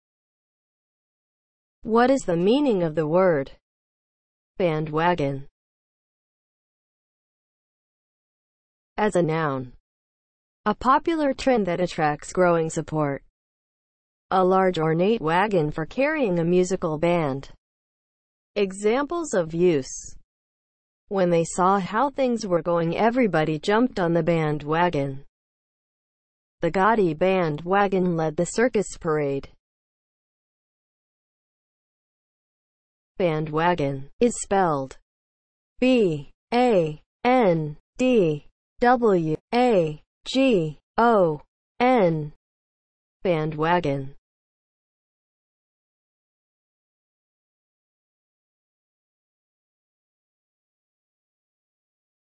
What is the meaning of the word BANDWAGON?
Definition, examples of use and spelling of the word BANDWAGON.

(00:00) Intro
(00:08) As a noun
(00:32) Spelling

As a noun (00:08):
• a popular trend that attracts growing support
• a large ornate wagon for carrying a musical band

Examples:
• "when they saw how things were going everybody jumped on the bandwagon"
• "the gaudy bandwagon led the circus parade"